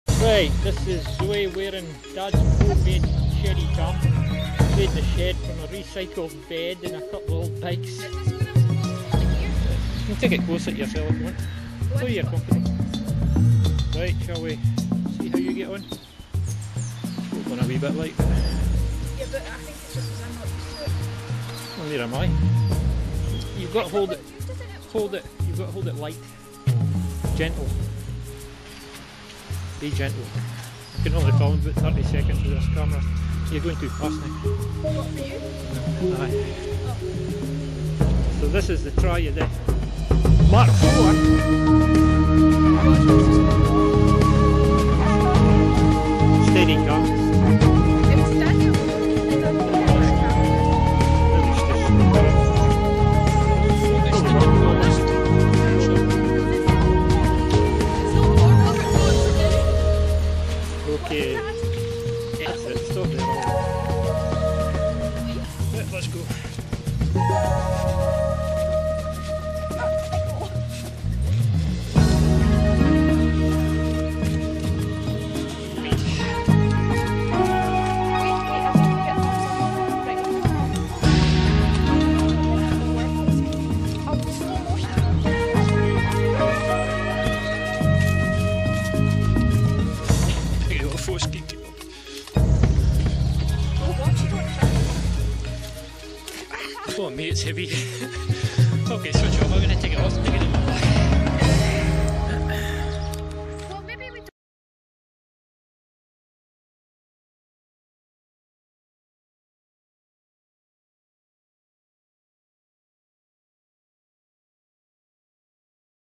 Shedy cam in action with Panasonic SD900
Home made camera stabiliser. Made from the frame and springs of a bed sofa and a couple of bike wheel hubs. Built in to a old rucksack I had,  99% recycled. Only bought nut, bolts, bearings and a mount for the camera. Made to be use with the Panasonic SD900.
About 200 hours went in to this through 4 versions till I got it to work and I think that was by accident. LOL